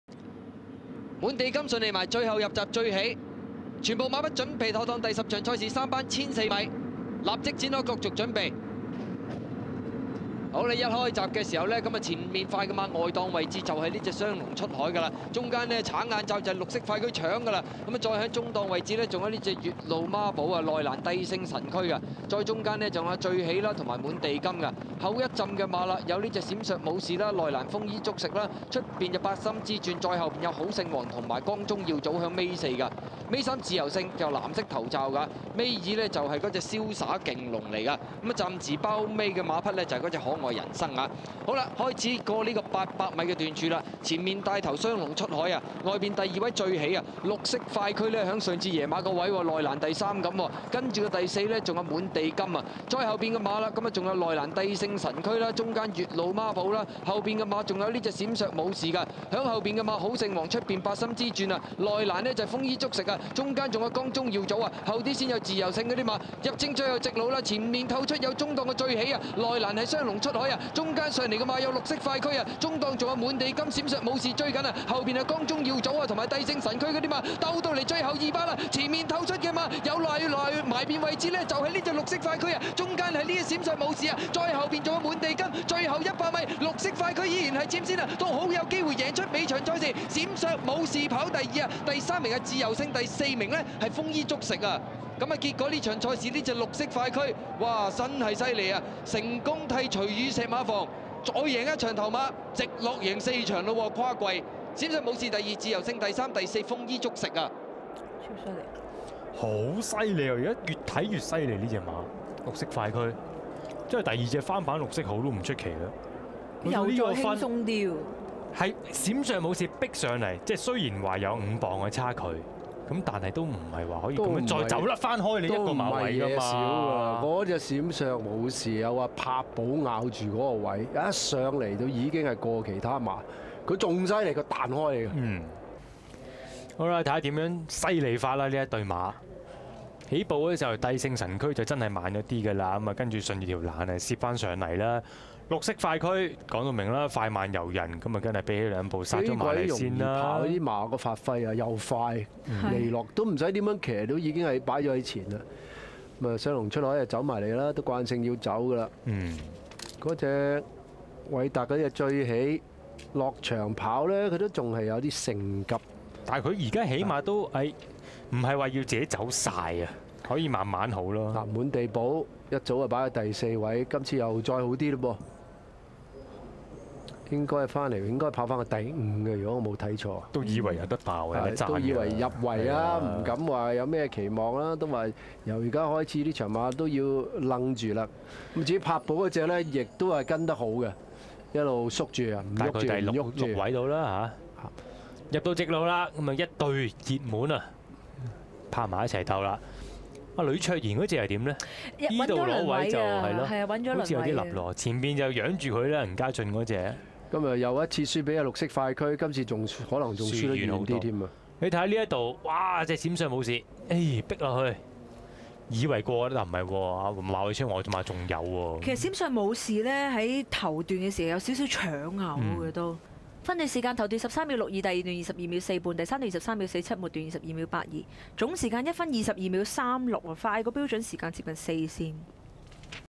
10/11/2012 (157) 綠色快駒 (N052) 馬偉昌
10/11/2012 沙田 (場次157)
第三班 - 1400米 - (85-60) Panasonic Lamdash電鬚刨讓賽
場地狀況 :  好至快地
賽道 :  草地 - "A"
分段時間 :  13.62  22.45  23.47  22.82